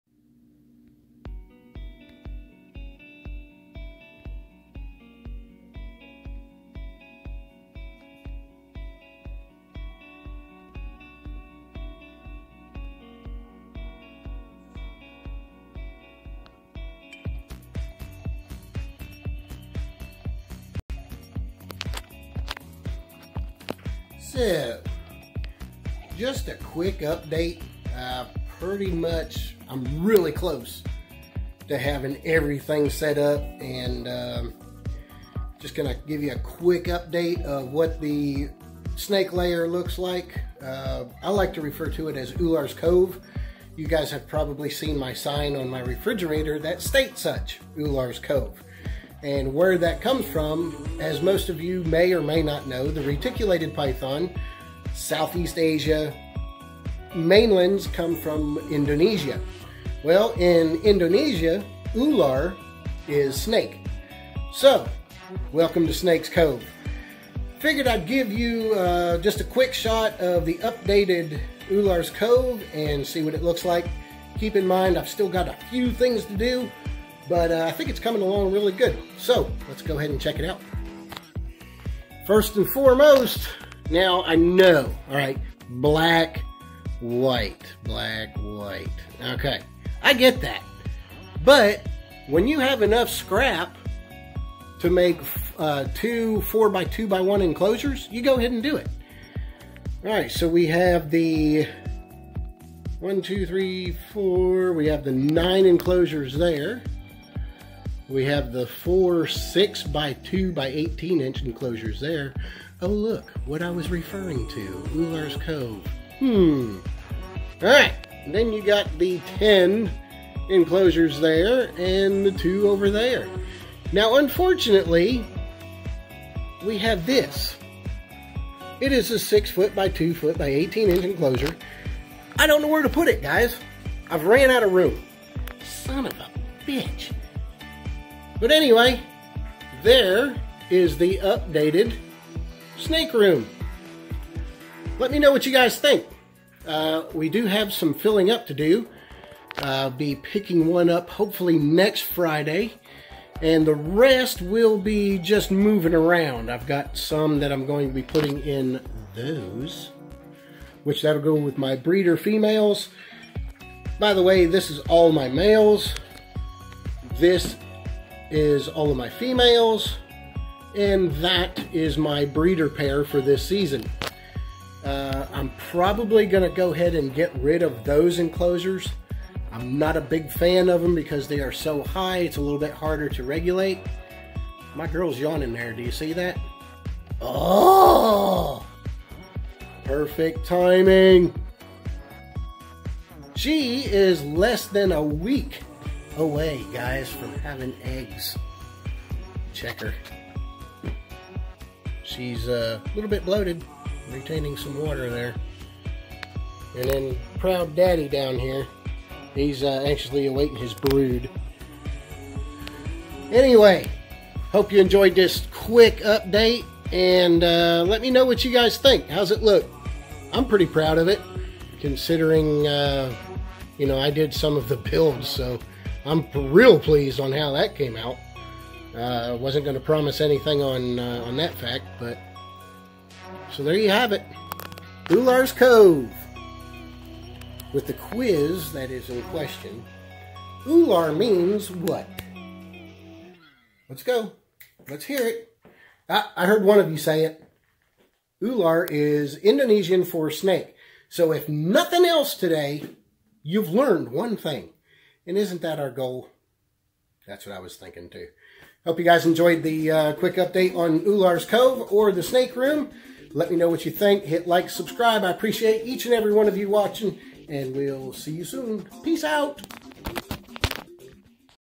Quick "Near Complete Snake Lair" Tour.
Catch a glimpse of the almost completed Ular's Cove. With nearly 90% complete, it's all coming together beautifully and as planned. 😁 Let me know what you think. Snake Room Tour, Snakes, Pythons, Reptiles, My Own Zoo, My Dream Coming True.